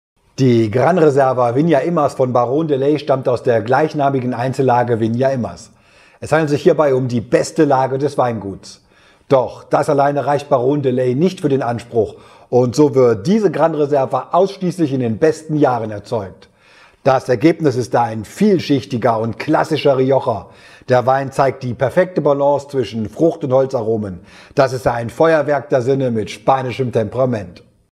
Baron de Ley Gran Reserva Vina Imas DOC
Der Baron de Ley Gran Reserva Vina Imas ist ein Rotwein aus Spanien. aus der Region Rioja.